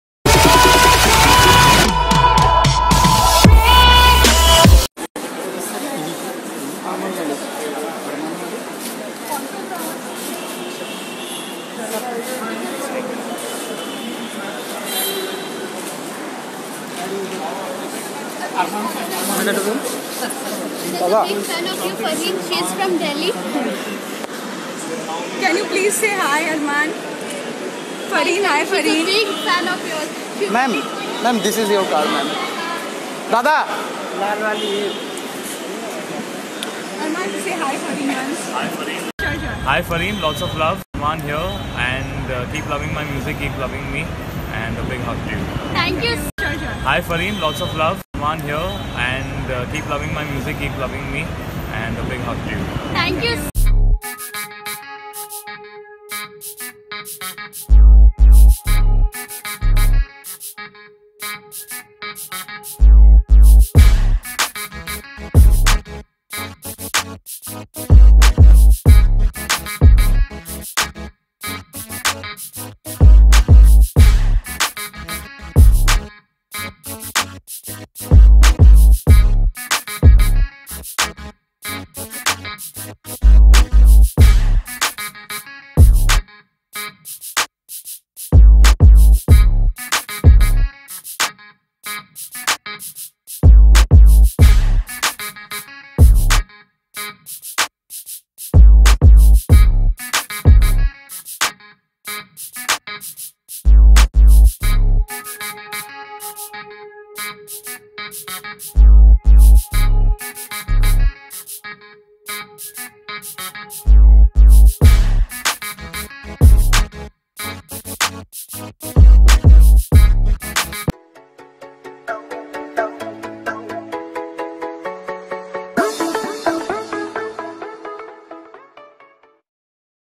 Armaan Malik Landed Kolkata - & Taken Pictures with Armaanians || Fan Moments || 2018
Armaan Malik Landed Kolkata For NRS Medical College Festival Concert 2018 & Taken Pictures with Armaanians with Few Amazing Fans Moments 2018.